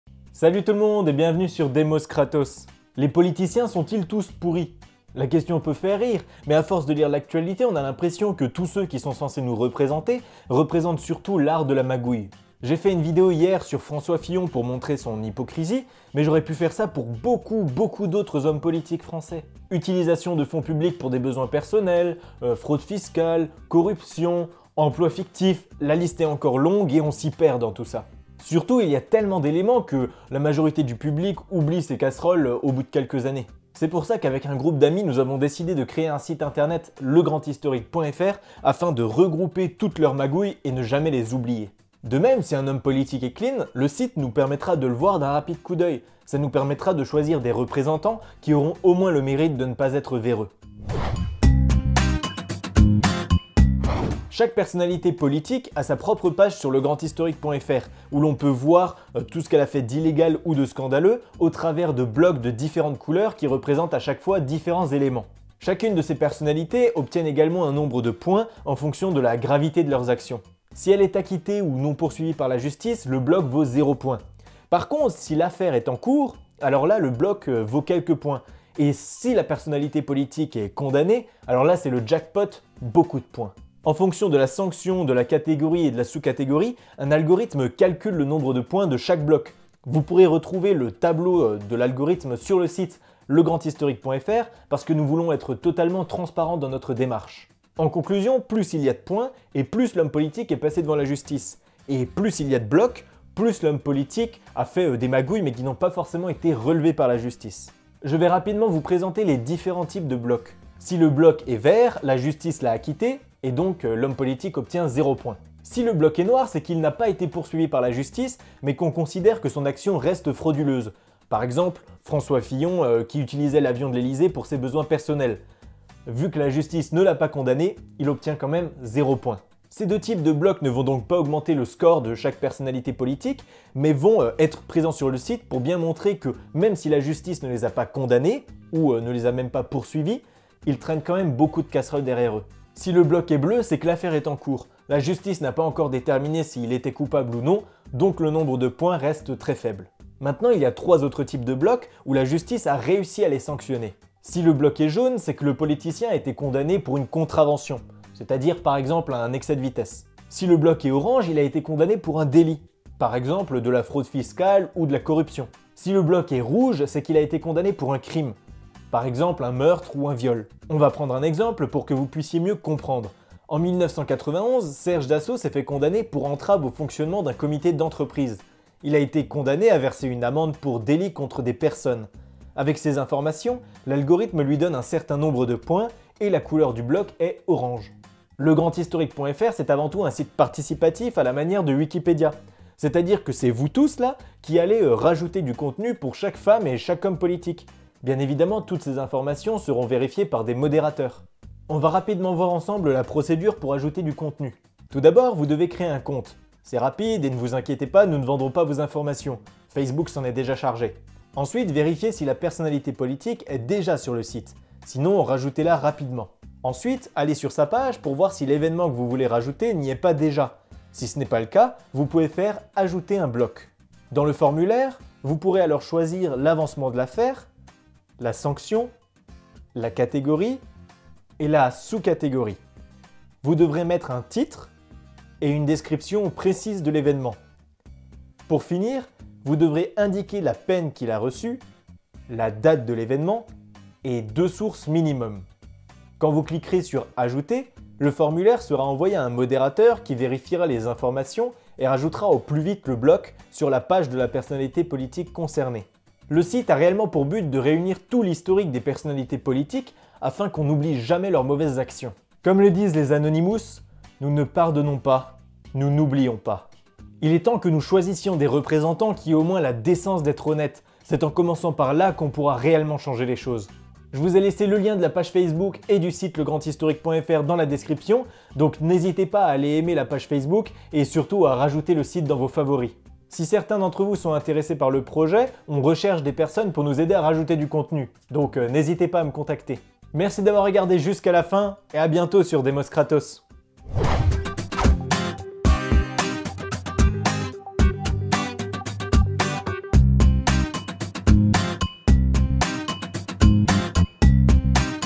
Les politiciens sont-ils tous pourris ?
Les politiciens sont-ils tous pourris ? J'ai un moyen rapide de le vérifier à vous présenter !

Si vous voulez participer à ce site collaboratif, n'hésitez pas à me contacter ! On recherche du monde !

4885